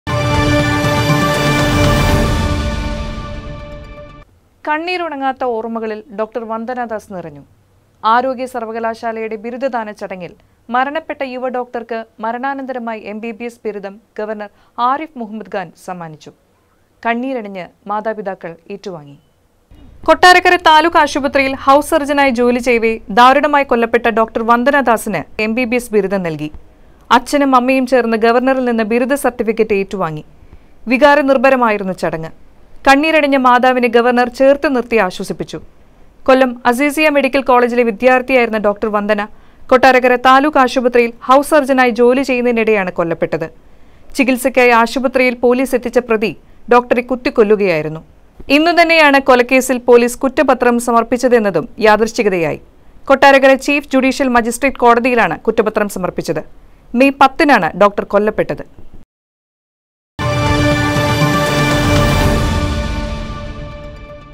കണ്ണീരുണങ്ങാത്ത ഓര്‍മ്മകളില്‍@newsleaderchannel
ആരോഗ്യസര്‍വ്വകലാശാലയുടെ ബിരുദദാന ചടങ്ങില്‍ മരണപ്പെട്ട യുവഡോക്ടര്‍ക്ക് മരണാനന്തരമായി എംബിബിഎസ് ബിരുദം ഗവര്‍ണ്ണര്‍ ആരിഫ് മുഹമ്മദ് ഖാന്‍ സമ്മാനിച്ചു. കണ്ണീരണിഞ്ഞ് മാതാപിതാക്കള്‍ ഏറ്റുവാങ്ങി. കൊട്ടാരക്കര താലൂക്ക് ആശുപത്രിയില്‍ ഹൗസ് സര്‍ജനായി ജോലിചെയ്യവേ ദാരുണമായി കൊല്ലപ്പെട്ട ഡോ. വന്ദന ദാസിന് മരണാനന്തര ബഹുമതിയായി എം ബി ബി എസ്സ് ബിരുദം നല്‍കി. അച്ഛനും അമ്മയും ചേര്‍ന്ന് ഗവര്‍ണറില്‍ നിന്ന് ബിരുദ സര്‍ട്ടിഫിക്കറ്റ് ഏറ്റുവാങ്ങി.